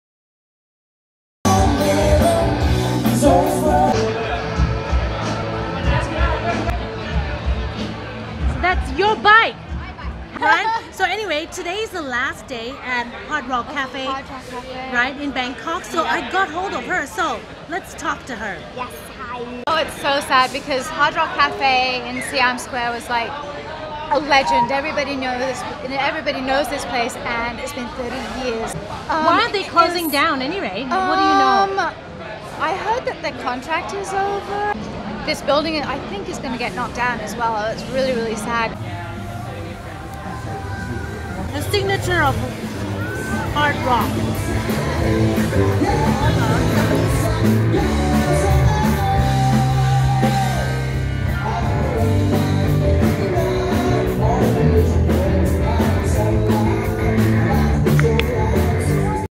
(แคทรียา อิงลิช) explains the shut down of Hard Rock Cafe Bangkok
"Dear Hard Rockers," the café wrote on the Facebook post, "Since opening the doors of Hard Rock Café Bangkok in March 1991, we have enjoyed playing a major role in the city’s dining and music culture for the past 30 years.
On Wednesday, March 31st , 2021, Hard Rock Café Bangkok (Siam Square) will PAUSE and close its restaurant.
 This announcement comes across as shocking news to the city, as this rock music-forward establishment has been an iconic landmark in the area of Siam Square for three decades.
Hard Rock revealed in the post that the reason for this departure is the completion of 30-year lease at the very location.
No ETA of Hard Rock's come-back had been announced.